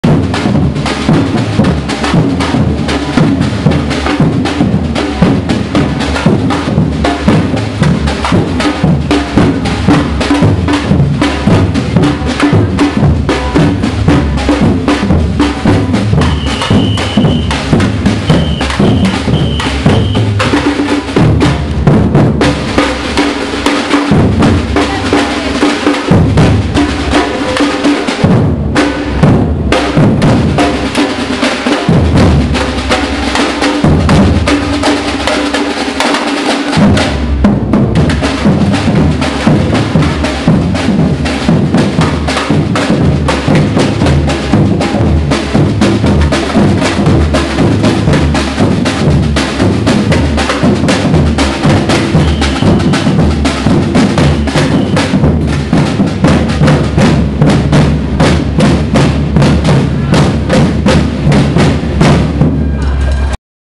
samba welcoming for Mr Wong Kan Seng
Welcoming for Mr Wong Kan Seng, Deputy Prime Minister & Minister for Home Affairs

At City Square Mall for the Central Singapore Clean & Green Singapore 2009 event.